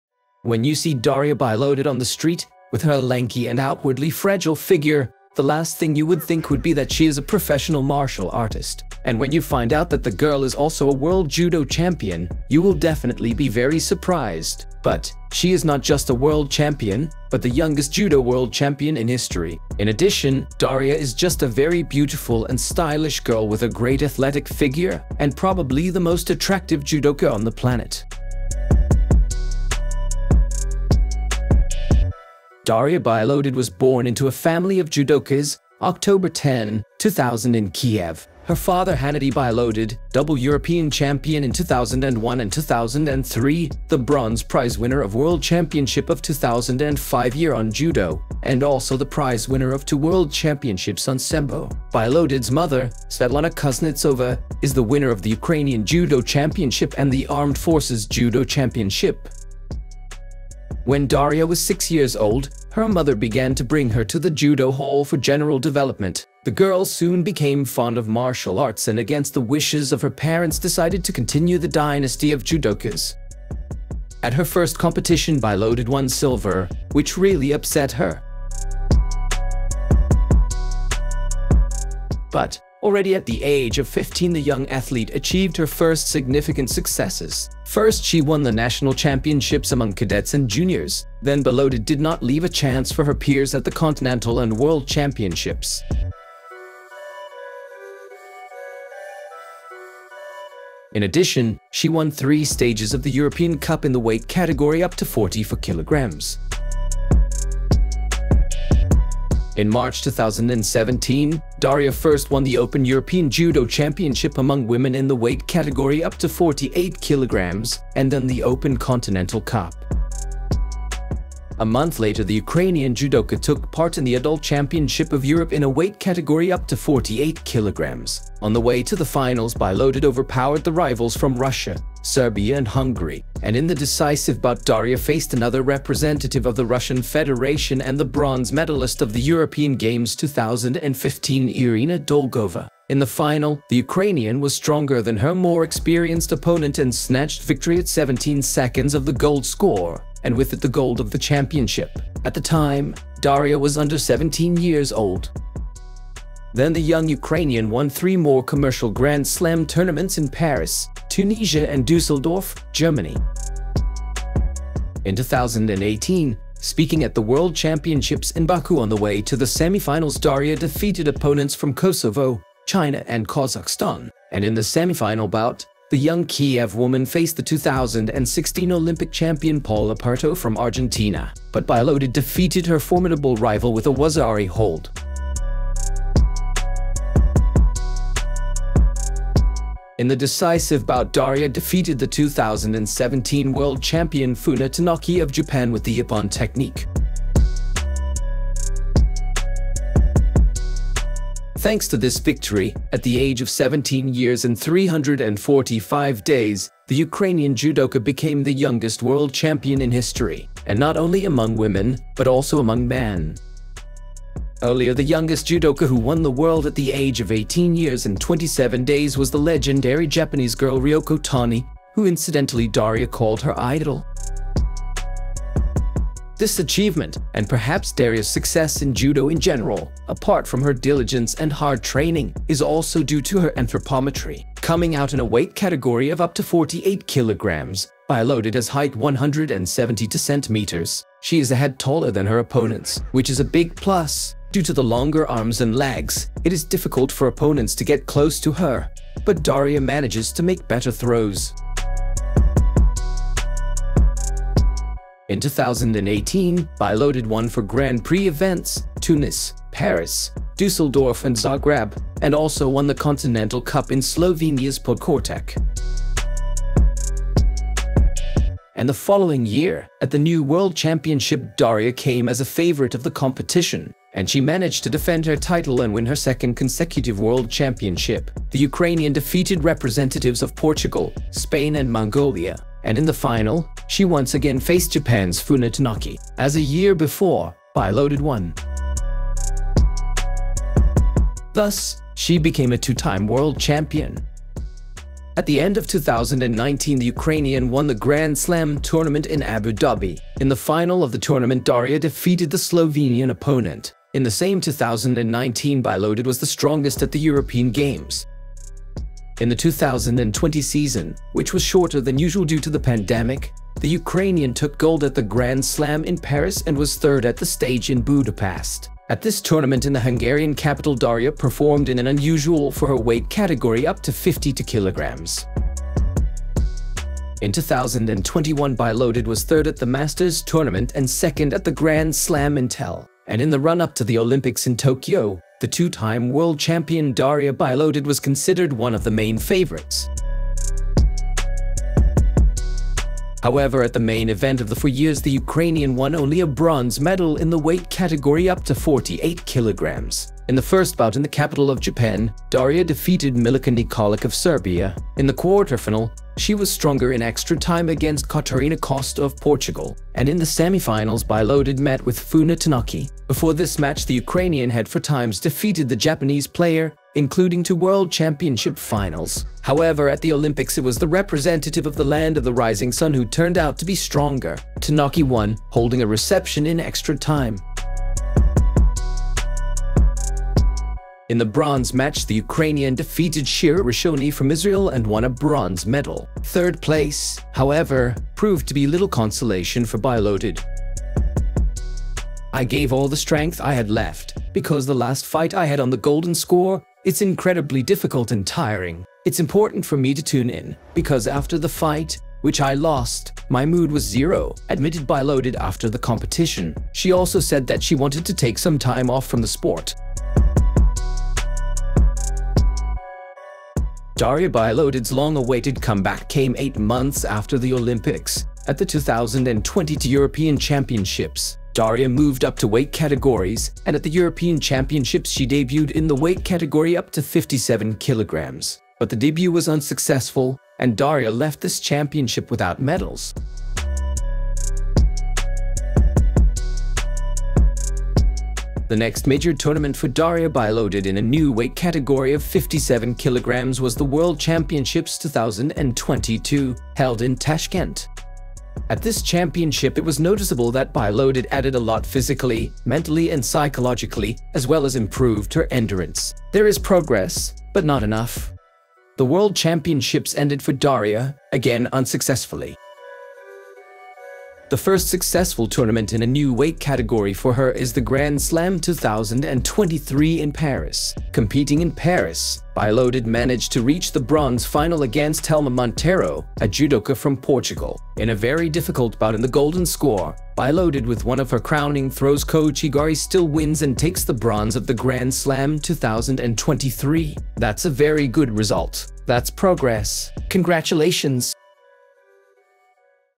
She's A Beast On The Tatami. The Most Beautiful Girl in Judo - Daria Bilodid
When you see Daria Bilodid on the street, with her lanky and outwardly fragile figure, the last thing you would think would be that she is a professional martial artist. And when you find out that the girl is also a world judo champion, you will definitely be very surprised. But, she is not just a world champion, but the youngest judo world champion in history. In addition, Daria is just a very beautiful and stylish girl with a great athletic figure, and probably the most attractive judoka on the planet.